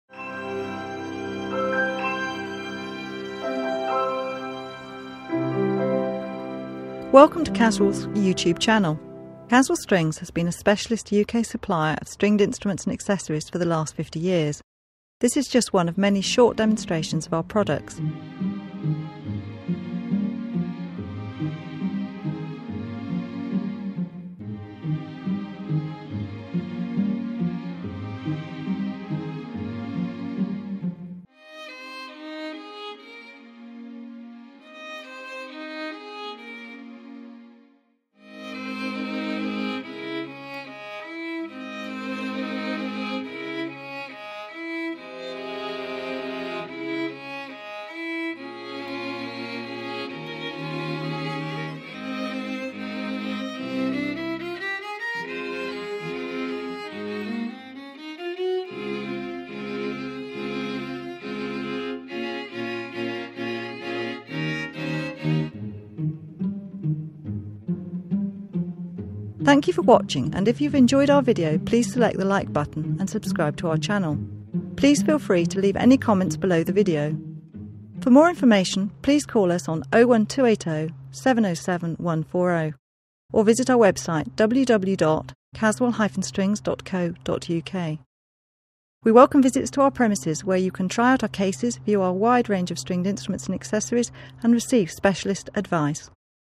Pedi Elegante Violin shoulder rest
Welcome to our new Shoulder Rest video series. This series will give you a good idea of the different shapes and sizes of shoulder rest available and also how to fit them to you Violin or Viola.